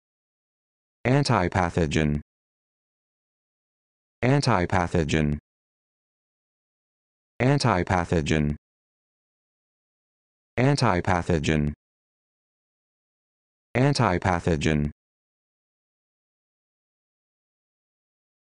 How to Pronounce Antipathogen
This video shows you how to pronounce Antipathogen